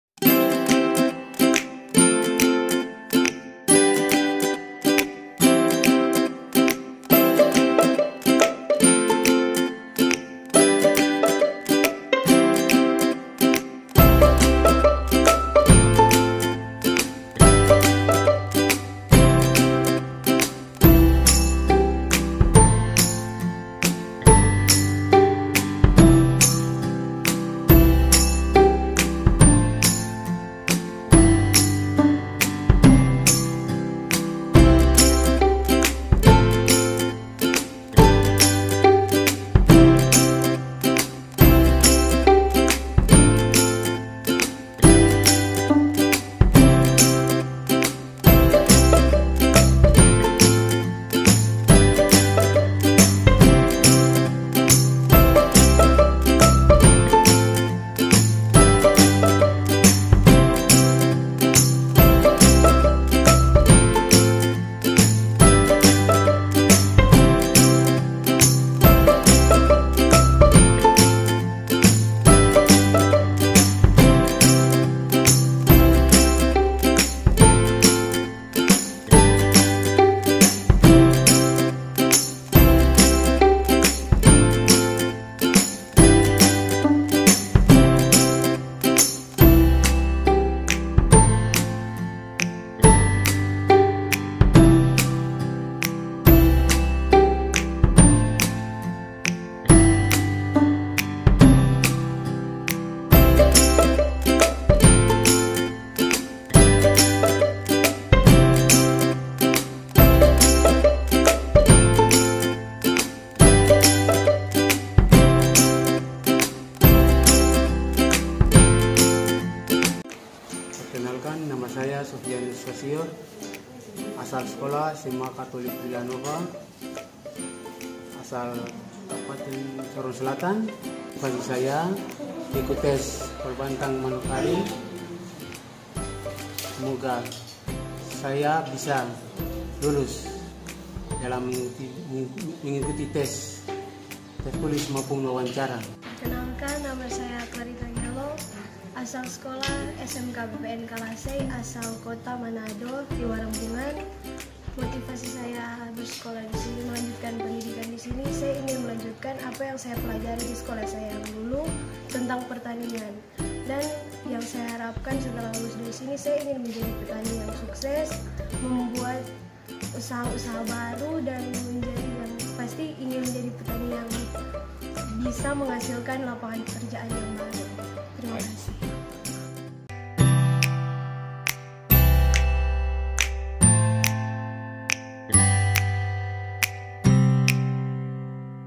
Penerimaan Mahasiswa Baru Polbangtan Manokwari Tahun 2018
Rangkaian Kegiatan mahasiswa baru Polbangtan Manokwari Tahun 2018 di Kampus Poteknik Pembangunan Pertanian Manokwari.